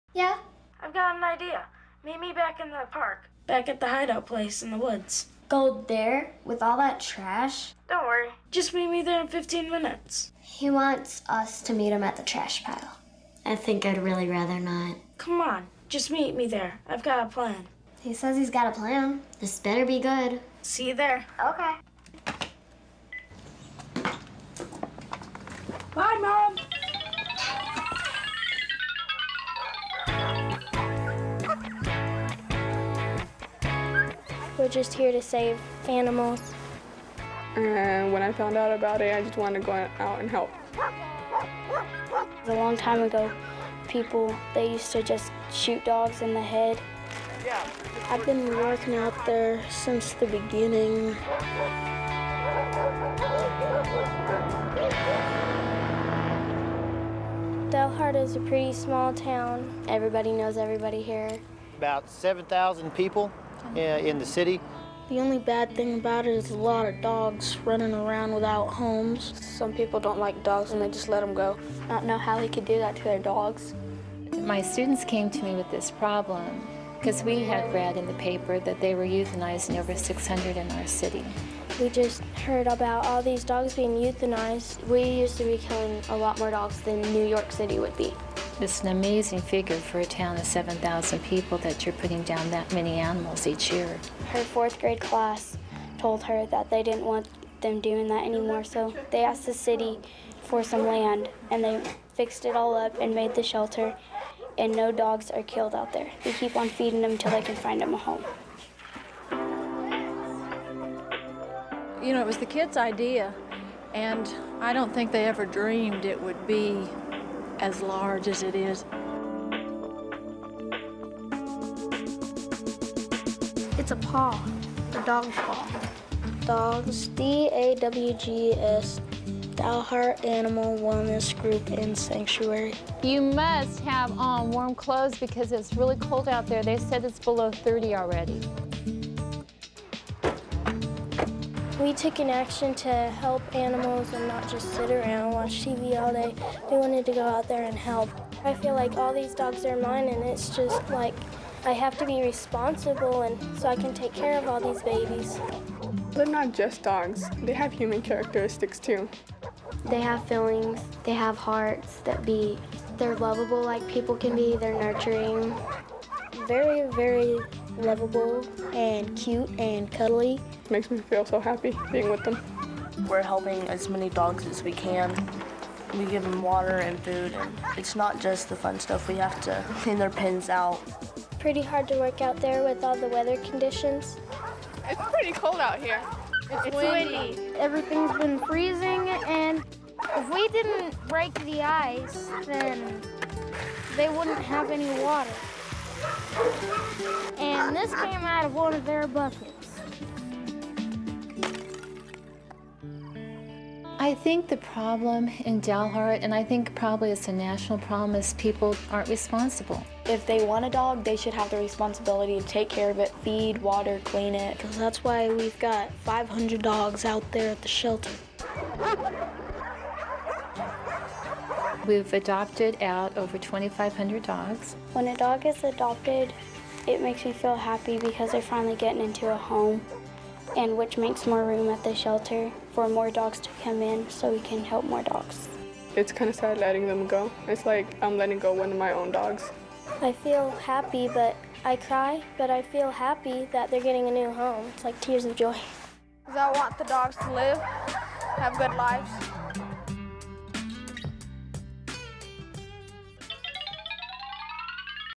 DAWGS: Dalhart Animal Wellness Group and Sanctuary
Courtesy of PBS: Diane Trull wasn't able to face the students of her fourth grade reading class and tell them that this is just how the world works -- that dogs must be killed just because there are too many of them, that they must be satisfied with this "adult" solution to the problem of pet overpopulation. She wasn't able to tell
the children that there was nothing they could do. And so began DAWGS -- the Dalhart Animal Wellness Group and Sanctuary. Fighting against overwhelming odds and obstacles posed by both man and nature, Diane and
her fourth graders set out to change "how the world works".